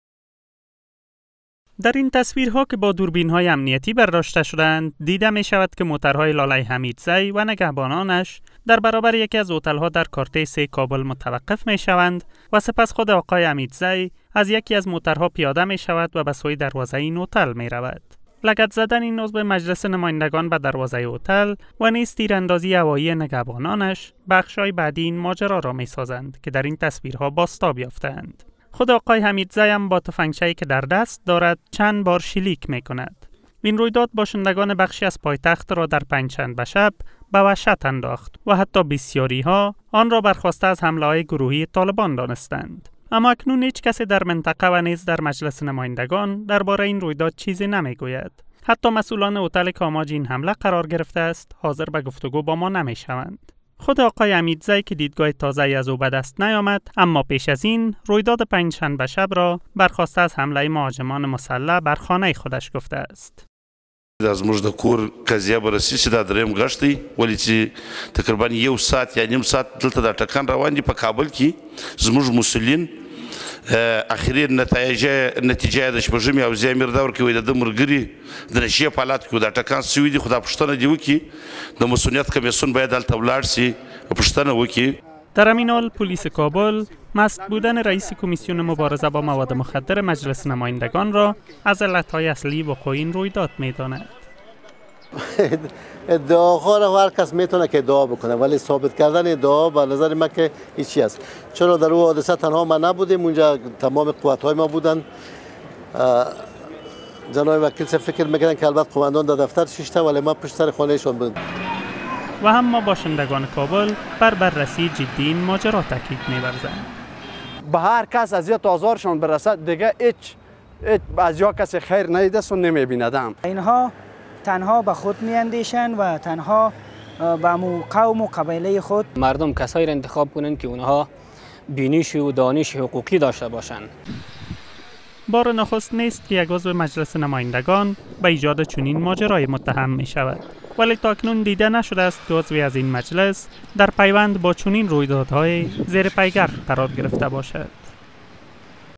Kabul Police to Probe Shooting Incident Involving MP
بررسی تیراندازی برهوتل از سوی لالی حمیدزی در دستور کار پولیس کابل